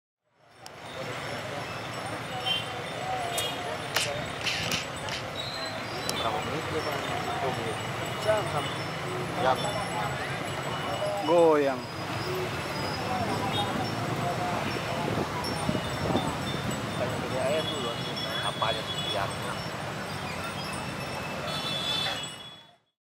Crocodile Lives With Tyre Around Its Neck
ID: 1462372

A crocodile is still living painfully with a scooter tyre around its neck two years after it was first spotted with it in a river in Indonesia.

The poor reptile was first spotted by locals in Palu in  in 2016 when it had headed to the riverbank with the scooter tyre around its neck, gasping for air.

The now 13-feet crocodile, believed to be the nearly 'virtually extinct' Siamese crocodile, was again spotted two weeks ago by locals when it had come to the riverbank, peeking out of water helplessly for air.

Seven volunteers are working to help the crocodile, supervised by Central Sulawesi Natural Resource Conservation Agency but their attempts have gone into vain.